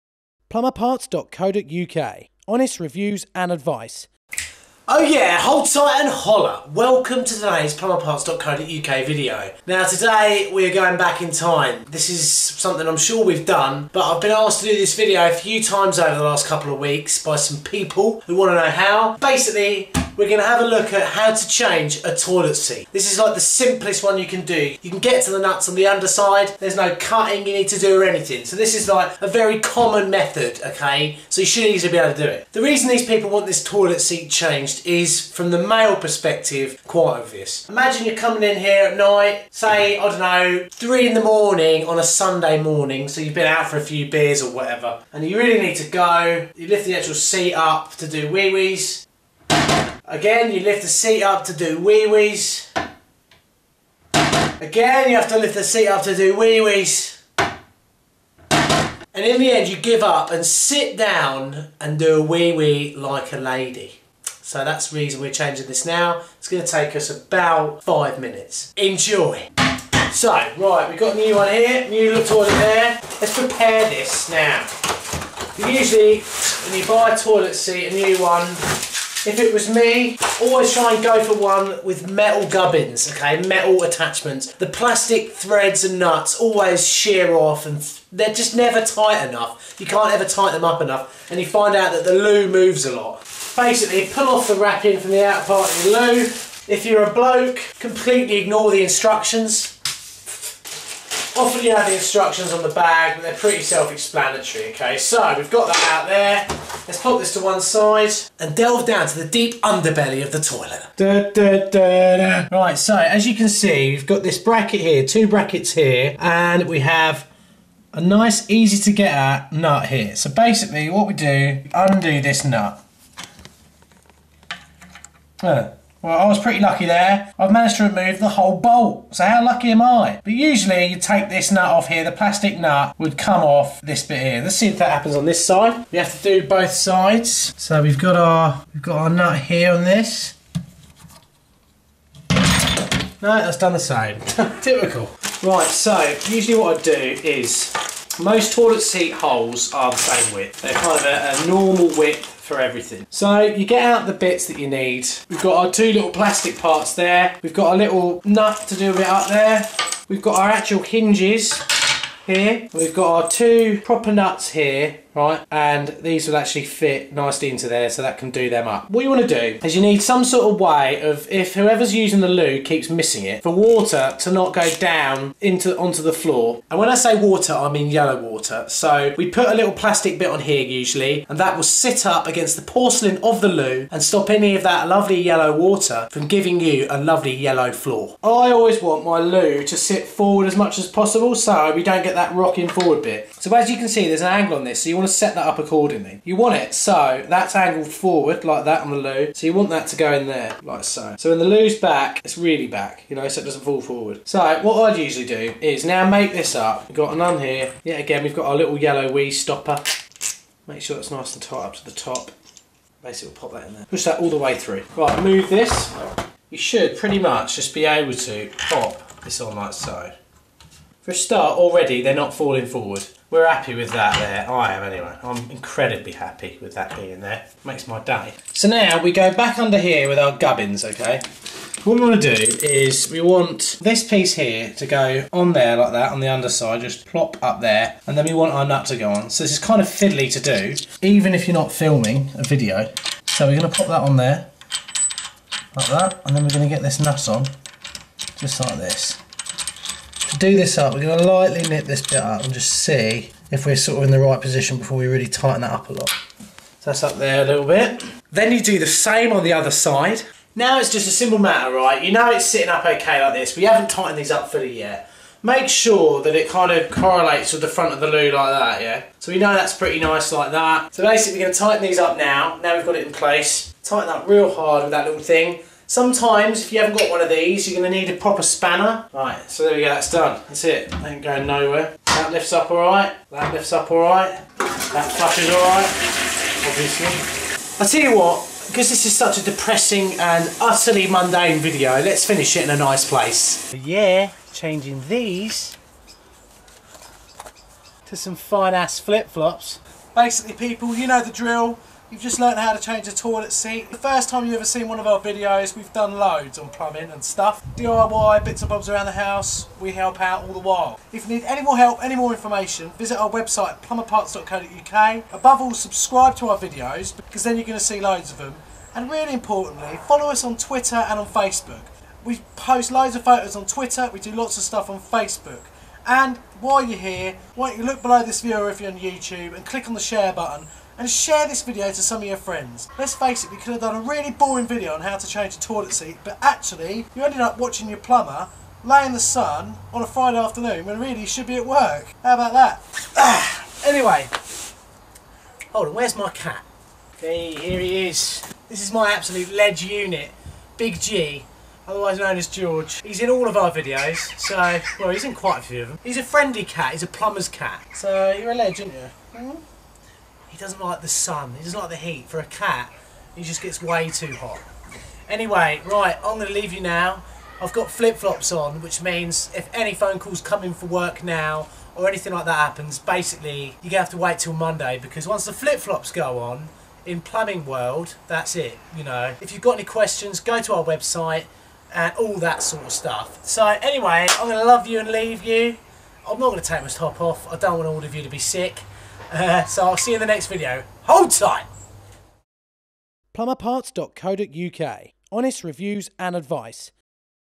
HOW TO CHANGE A TOILET SEAT - PLUMBING TIPS
This video is a step by step guide on how to change a toilet seat. From removing the old one without breaking the toilet to installing the new one in the right position. This also includes a slow closing toilet seat. We've tried to make it as fun as possible yet informative...enjoy!